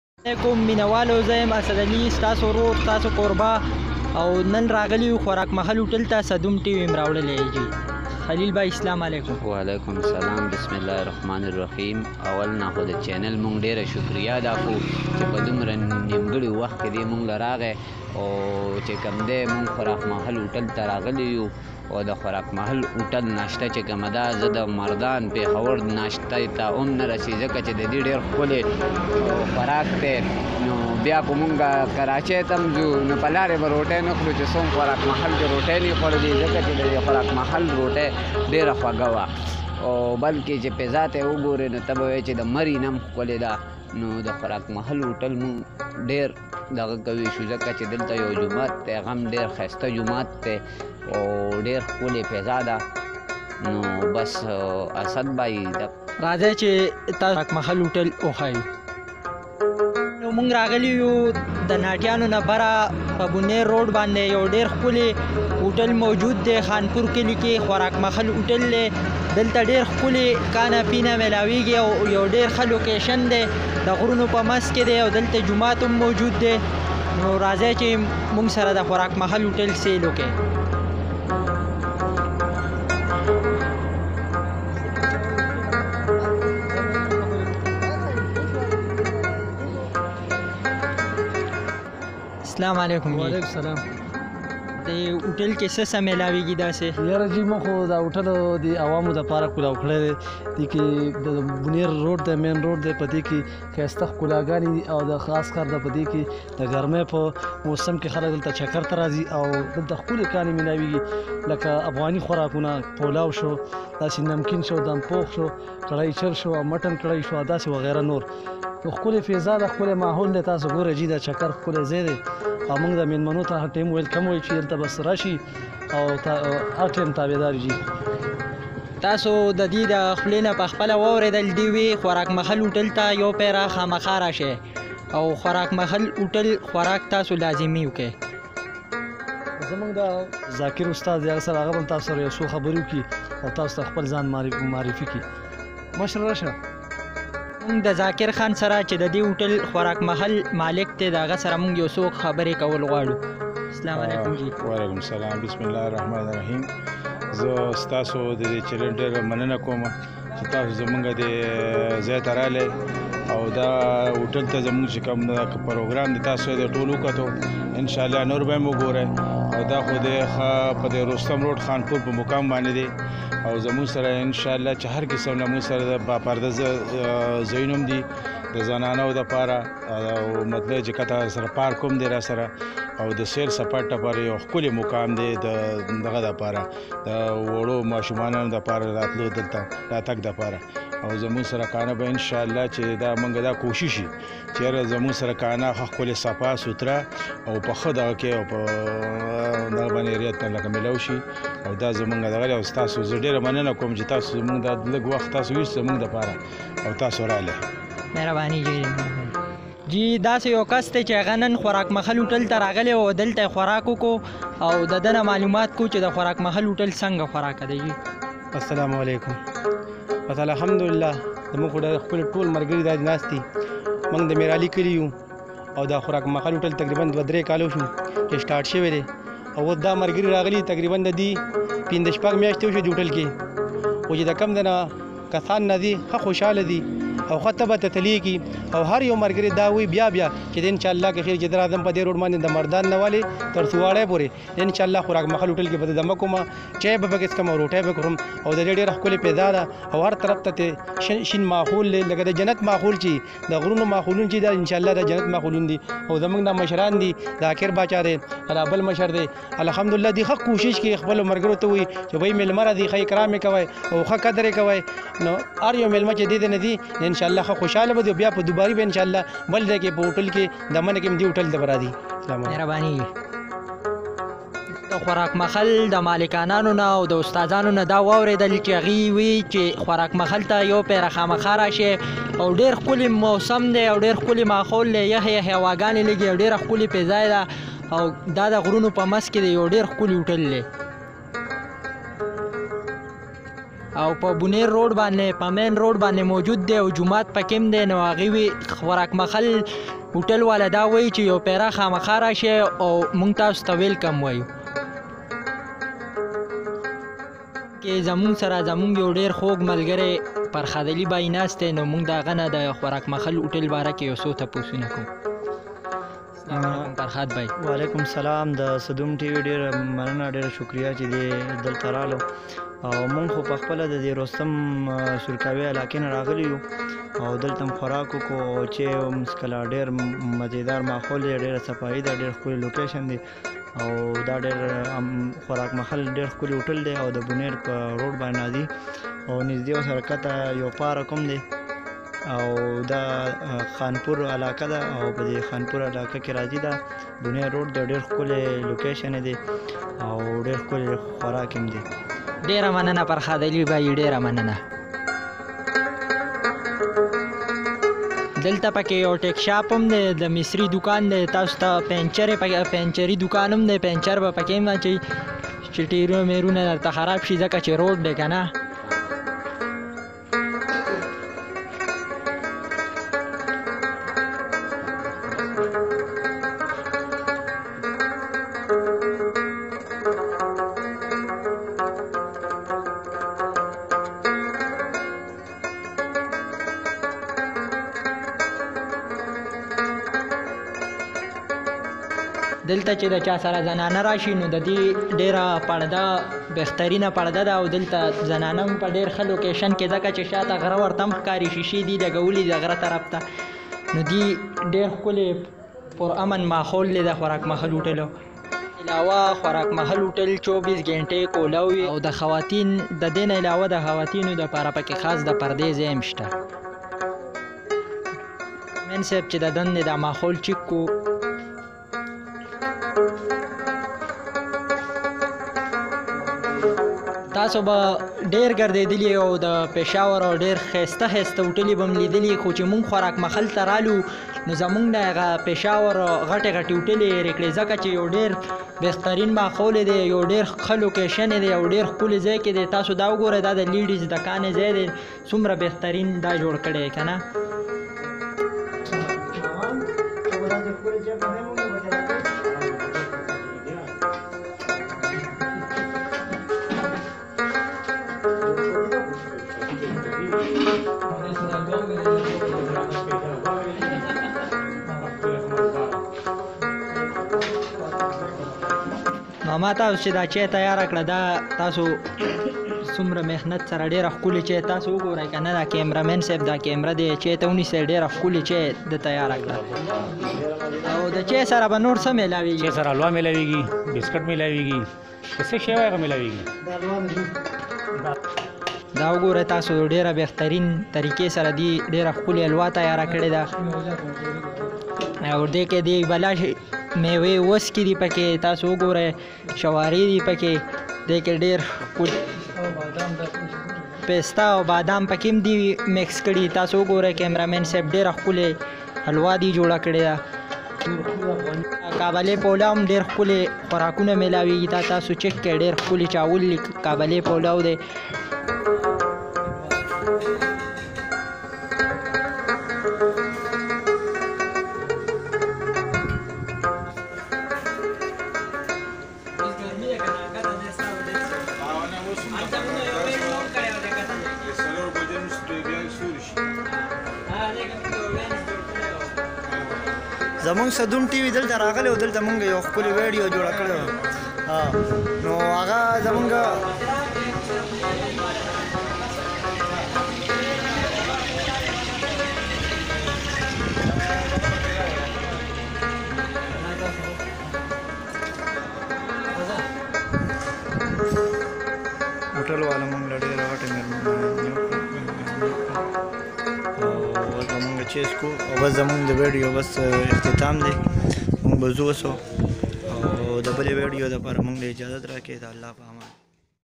خوراک محل ہوٹل خانپور ناوے ڈنڈ
khurak Mahal Hotal Khan Poor Nawy Dand

Pub G Ameer Part-2
Comming Soon.

Host:
ASAD Ali

Cameraman:
FARHAD ALI KHAN

Special Thanks:
Zakir Khan

Editor:
Farhad Ali

Director:
Farhad Ali